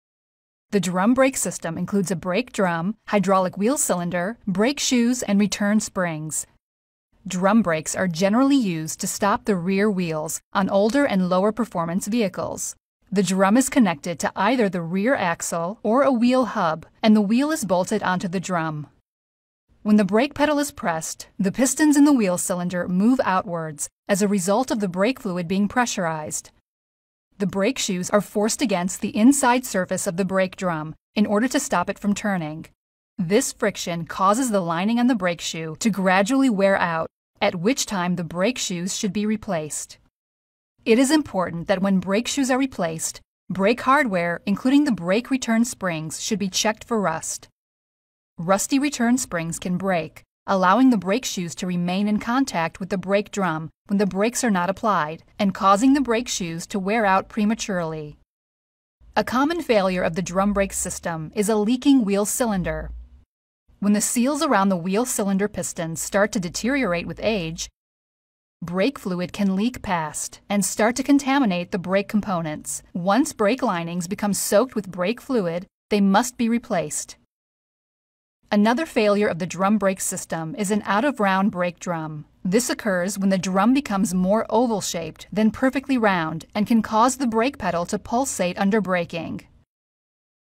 wheel cylinder - brake system
The drum brake system consists mainly of a hydraulic wheel cylinder, brake shoes, return springs, and a brake drum. Drum brakes are generally used to stop the rear wheels on trucks and lower performance cars. The drum is connected to either the rear axle or a wheel hub, and the wheel is attached to the drum. The wheel cylinder is the device in the top of the picture between the tops of the red brake shoes. Clicking Apply Brakes shows how the pistons in the wheel cylinder move outwards when the brake fluid is pressurized. This forces the brake shoes - the red/orange half circles on each side - to move outwards, forcing them to push against the inside of the brake drum and slowing then stopping the wheel from turning. The orange friction material on the brake shoes wears out as the brakes are applied. Removing the brake shoes involves dismantling the springs and clips that hold the red brake shoes in place. When installing new shoes it may be necessary to also replace the springs or hardware to keep the shoes in place properly.